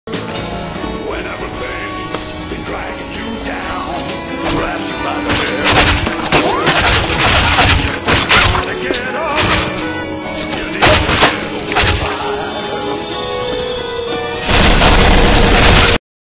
(ZARP:modded) what
im sure he is not mod or admin because when i check it no admin in this server that mean he is exploiter or he just join a group.